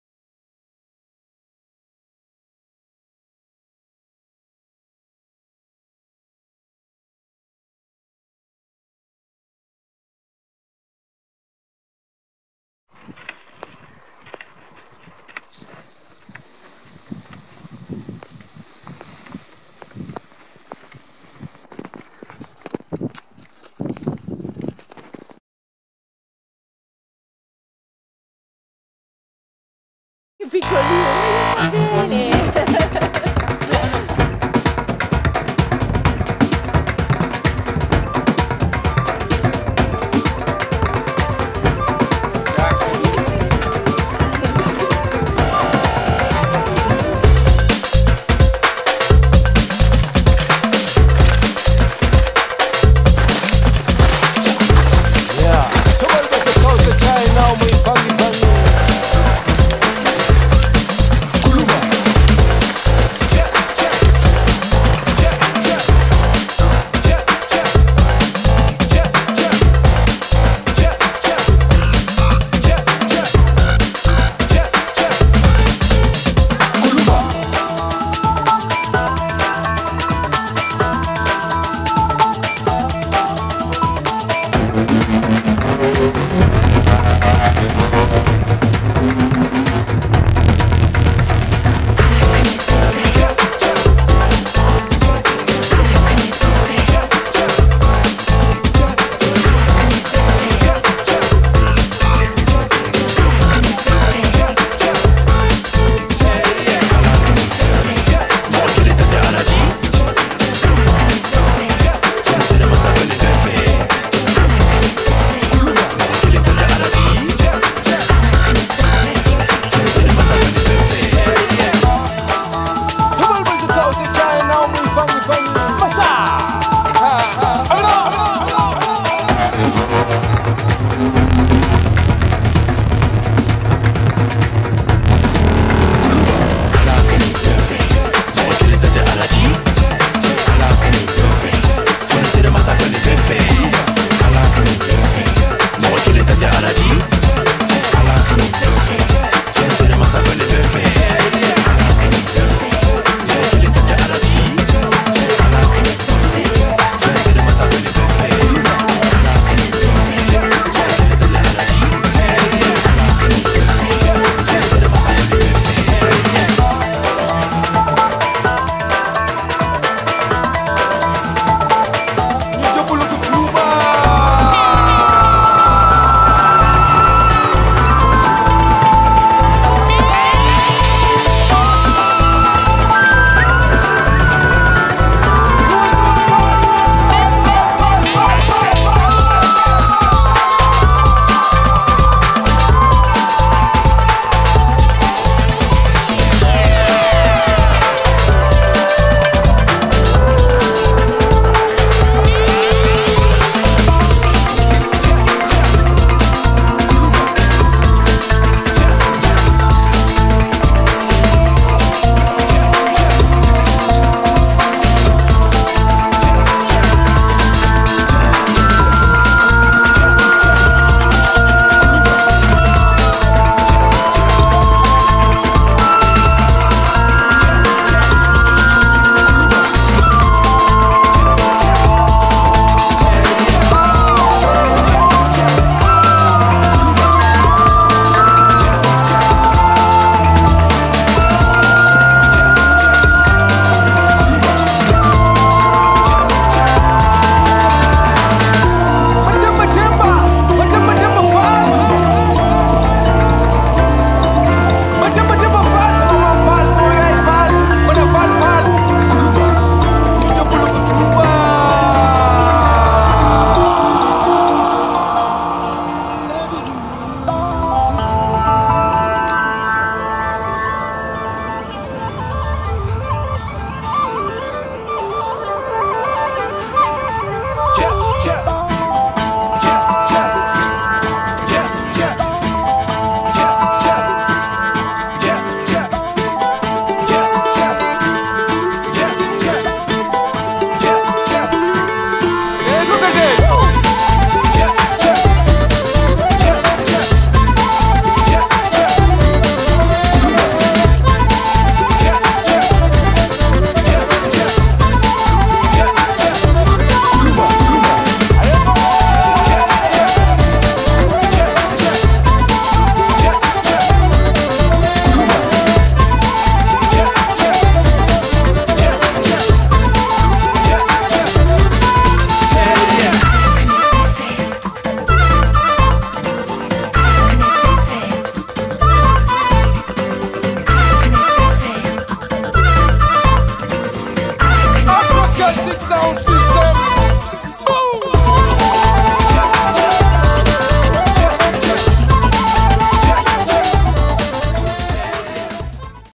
Sun & shadow. Scialpinismo in Dolomiti Forcella Cristallo
Scialpinismo in Dolomiti. Traversata da Passo Tre Croci alla Val Fonda per Forcella Cristallo.